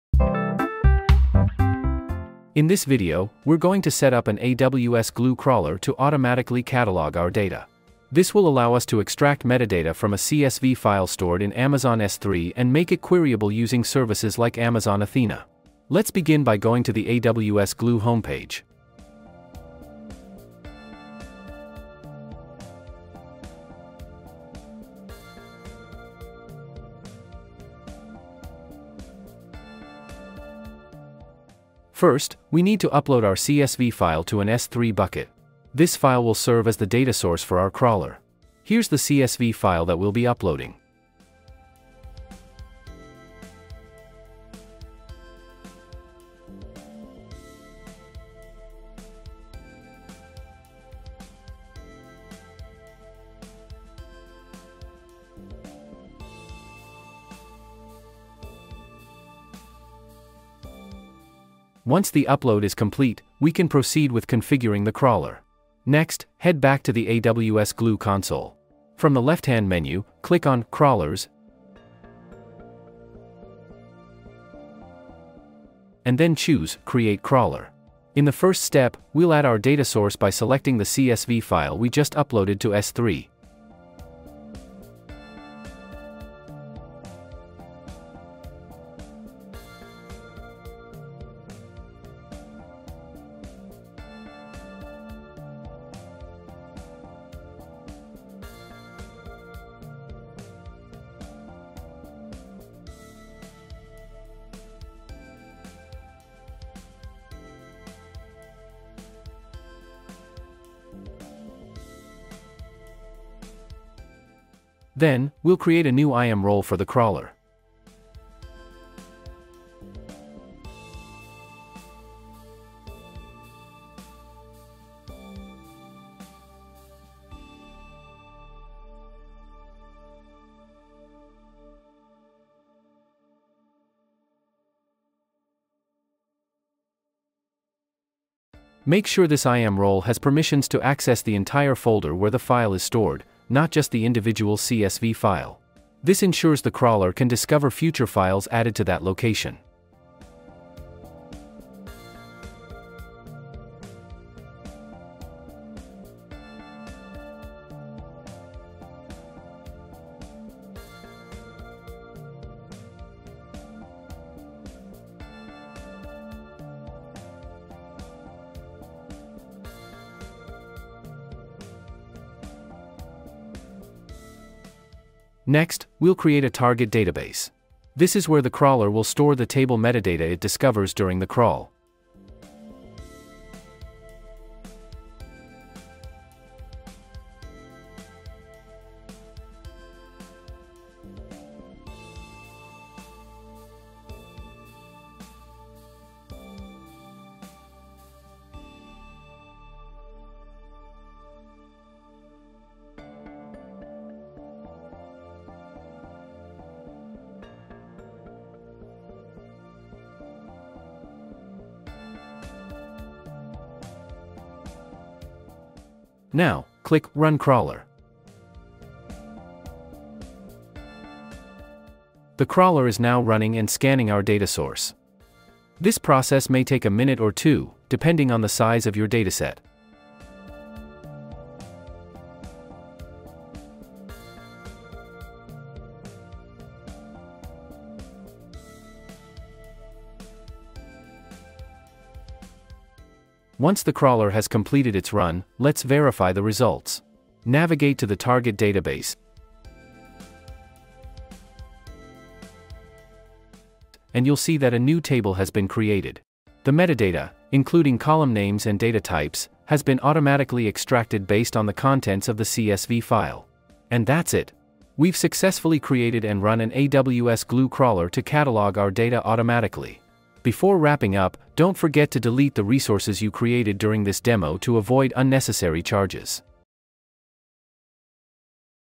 AWS Glue Tutorial: Automatically Catalog CSV Data from S3
Designing Data Repositories and Storage-Solutions for ML and DE

In this step-by-step tutorial, you’ll learn how to set up an AWS Glue Crawler to automatically catalog CSV data stored in Amazon S3 — a key skill for data engineering and machine learning workflows on AWS. AWS Glue helps you extract metadata, create tables, and prepare data for analysis with Amazon Athena — all without writing any code, making it an essential service to master for real-world data pipelines.

🎓 Perfect for AWS Certification Prep:
This hands-on walkthrough directly supports topics covered in the AWS Certified Data Engineer – Associate (DEA-C01) and AWS Certified Machine Learning – Specialty (MLS-C01) exams, especially in areas like data ingestion, transformation, cataloging, and query preparation.

🔍 What You’ll Learn:
Upload CSV data to S3 for use with AWS Glue
Create and configure an AWS Glue Crawler
Set up the IAM role with correct permissions
Create a target database for storing metadata
Run the crawler and verify the created table

By the end of this video, you’ll know how to automate data cataloging and integrate your data into analytics workflows — an essential step in building scalable data engineering and ML solutions on AWS.

⚠️ Important: AWS Glue is not free. You’re billed based on DPU usage and runtime, charged per second with a 1-minute minimum. If you want to avoid charges, simply watch the tutorial instead of following along. And remember to delete any resources created during this demo to avoid ongoing costs.

📚 More AWS Tutorials & Exam Prep:
👉 Explore our full collection of AWS certification books and study guides